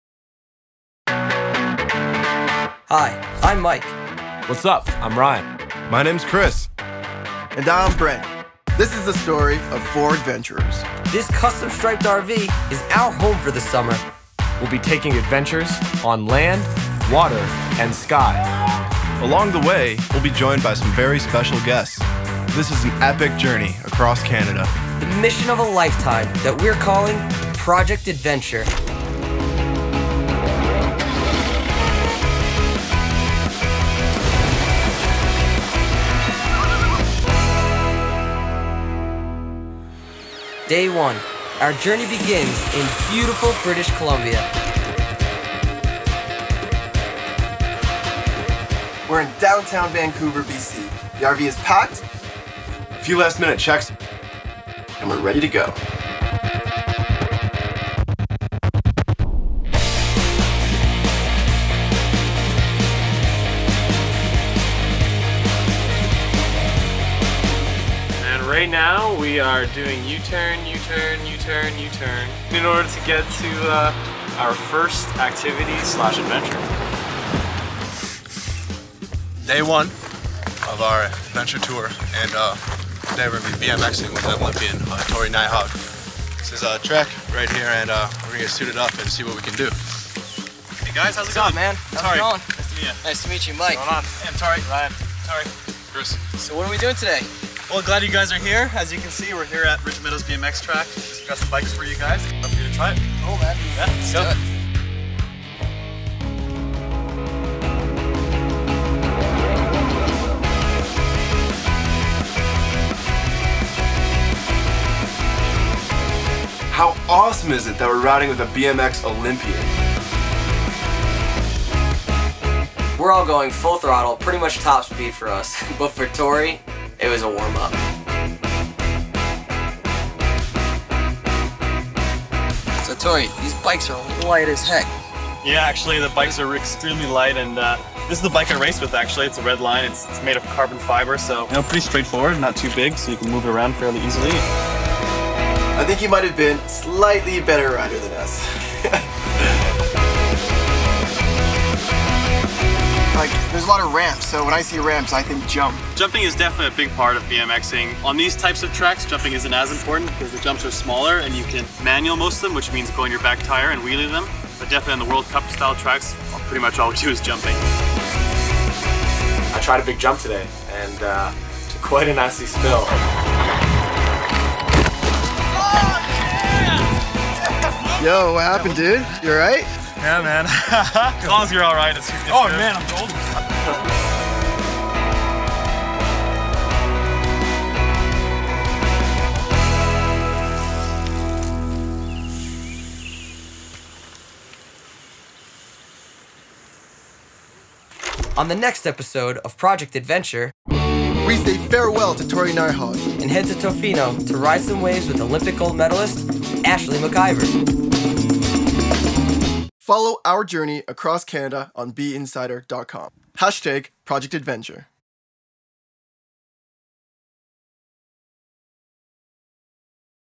Hudson's Bay ProjectAdventure - Episode 1
The adventurers start their journey by heading from downtown Vancouver to Pitt Meadows, BC to get a lesson in BMX riding from Olympic BMX athlete Tory Nyhaug. Hudson's Bay is setting out on their biggest adventure ever. Mike, Ryan, Chris and Brent are going an an epic cross Canada journey in a custom striped RV, stopping at some of the most exciting destinations the country has to offer!